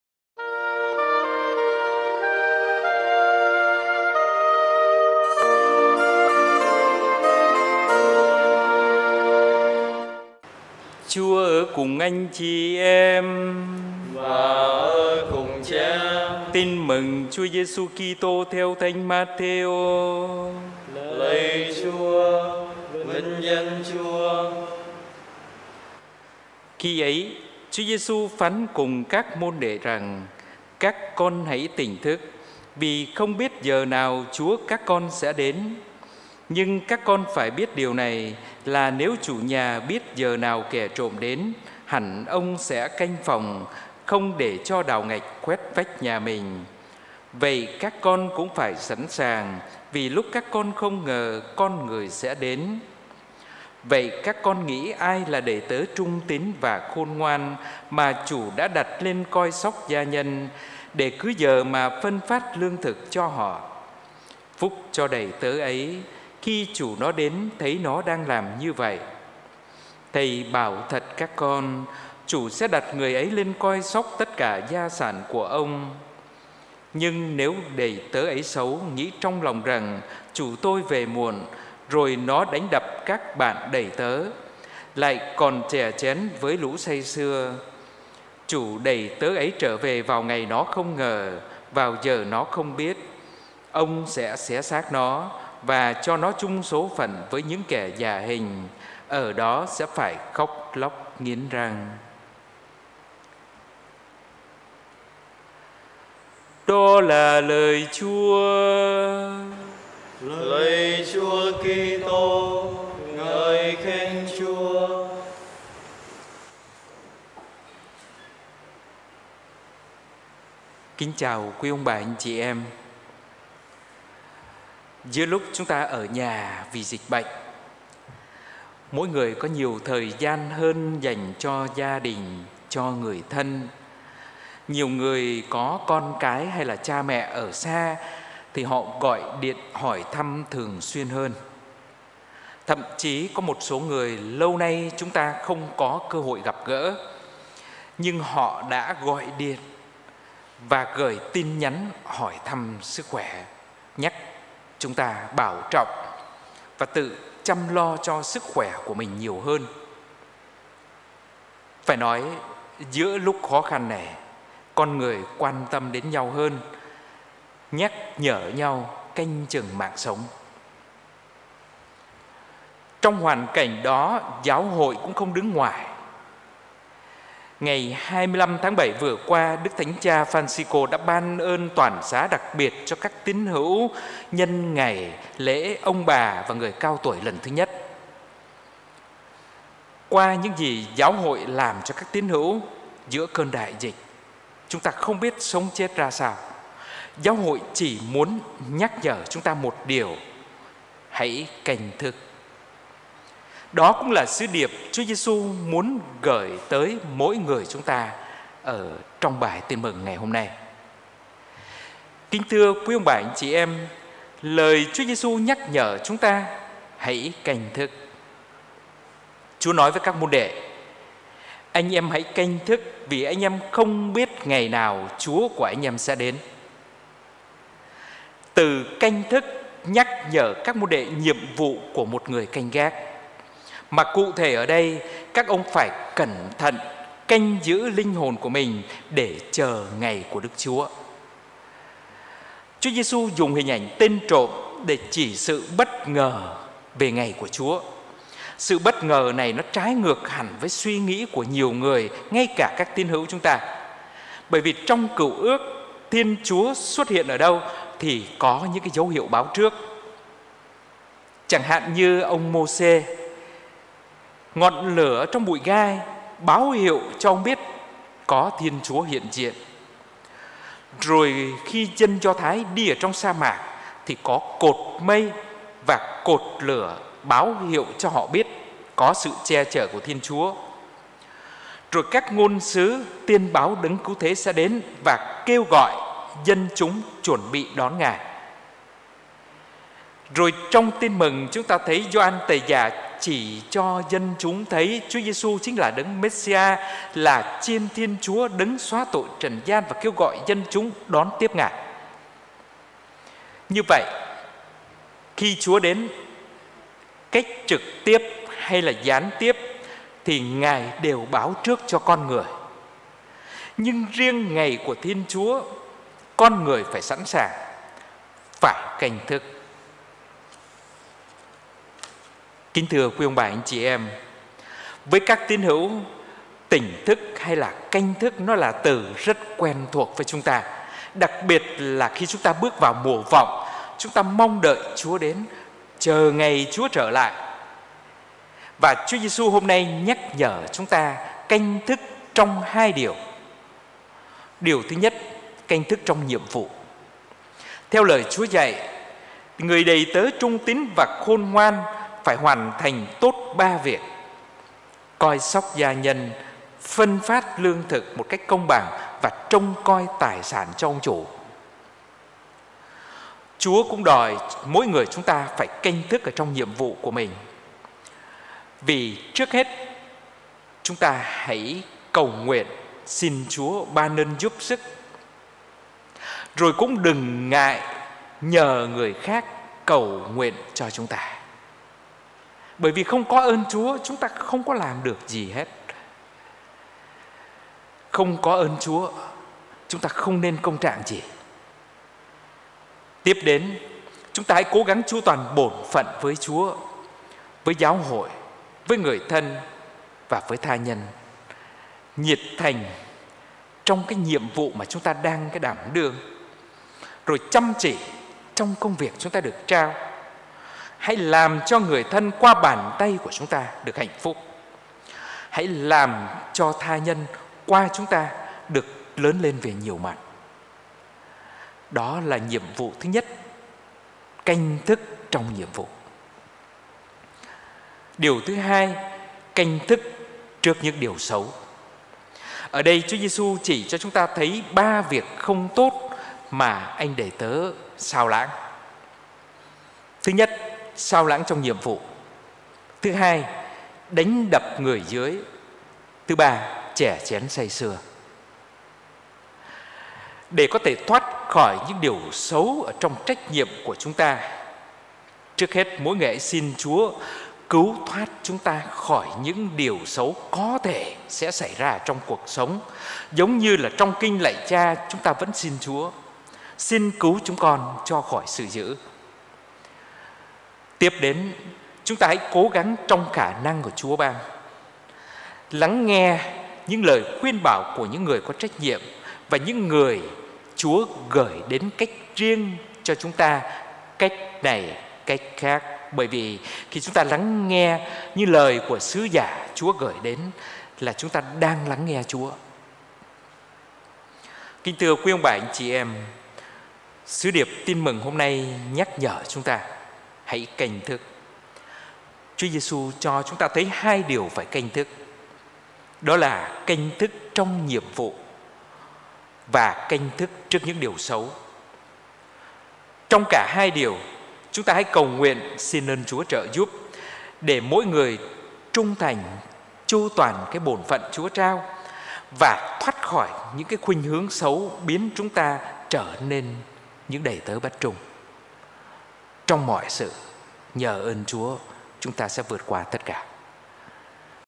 Bài Giảng Thánh LễTHỨ NĂM SAU CHÚA NHẬT XXI THƯỜNG NIÊN, Ngày 26/8/2021 - Gp. Cần Thơ
Thánh Lễ Trực Tuyến | THỨ NĂM SAU CHÚA NHẬT XXI THƯỜNG NIÊN | 17g00 | Ngày 26/8/2021 tại TTMV-GP. Cần Thơ
Kính mời quý vị  nhấn vào nút "Đăng ký" để có những video mới nhất.
Nếu Quý Vị thấy hữu ích, hãy chia sẻ video này !